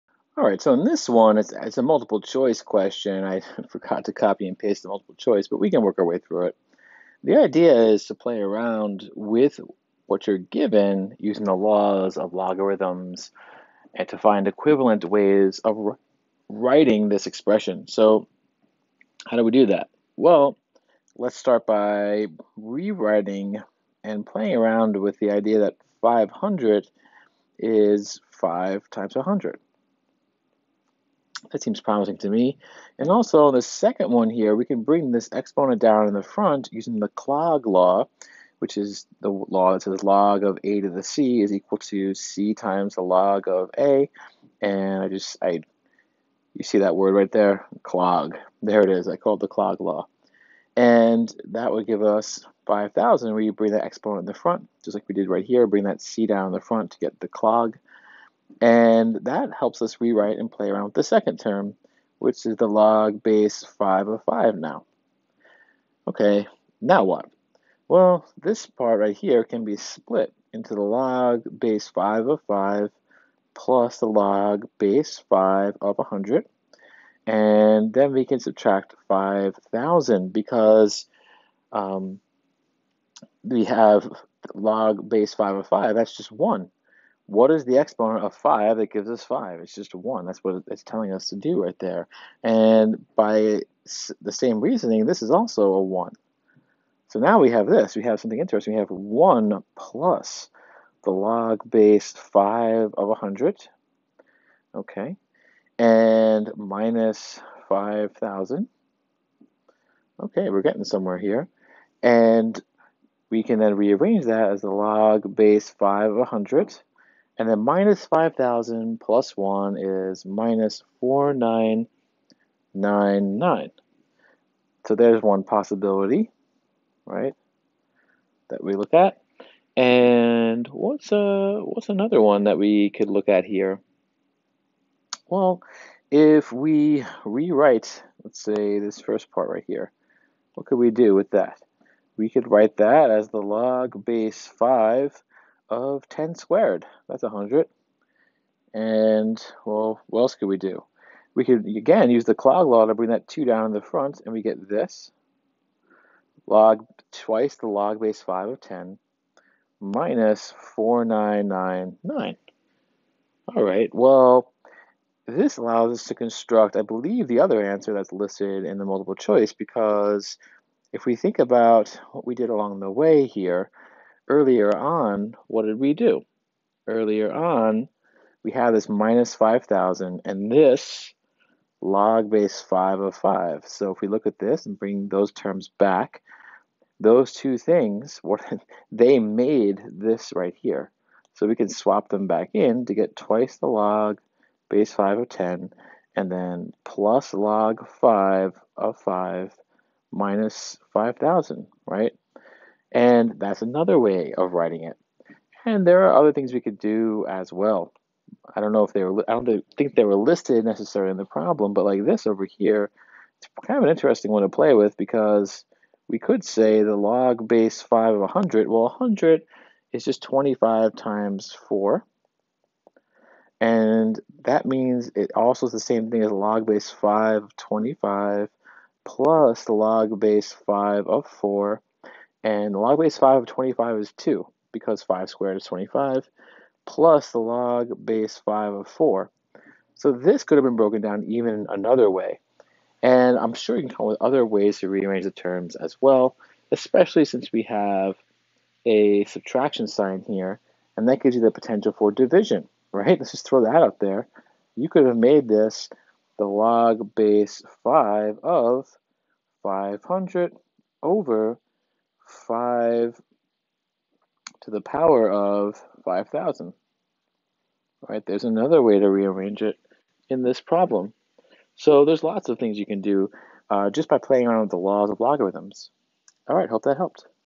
Log form 1
This is a worked example on Logarithms from Algebra 2